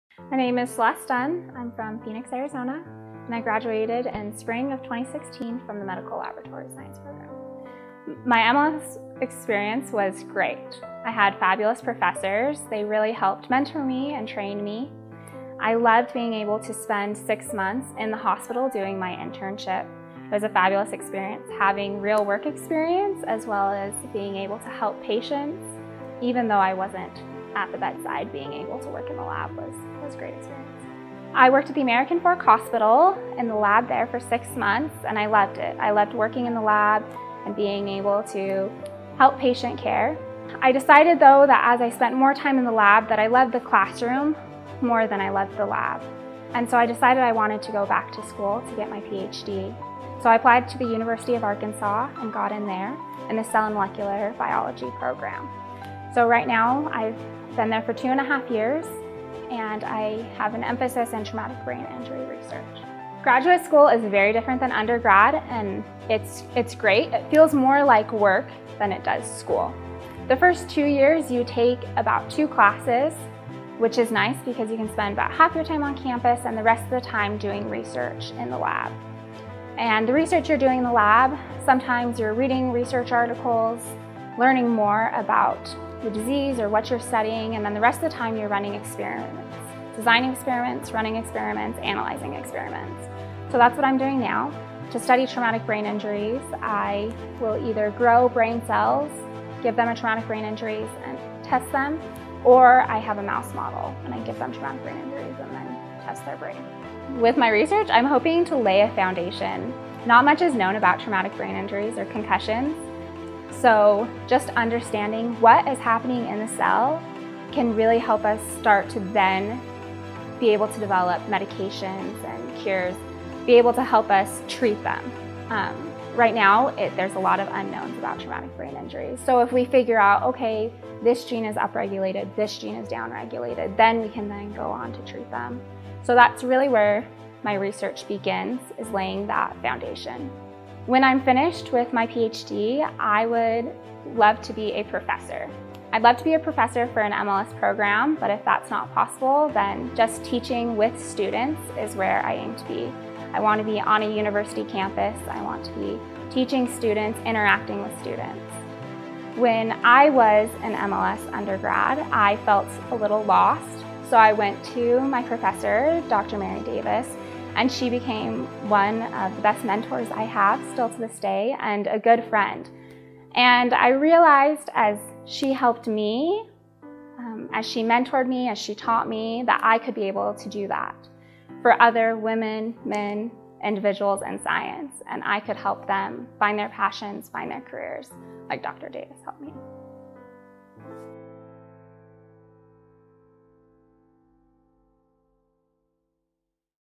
Celeste Dunn | MMBIO Alumni
Medical Laboratory Science alumnus Celeste Dunn tells us about her time in the major at BYU, and how it's shaped her post-graduate life.